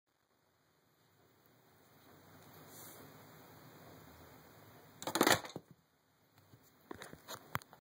My Xbox 360 mousetrap
I have too much time on my hands so i made a dead fall trap out of a 360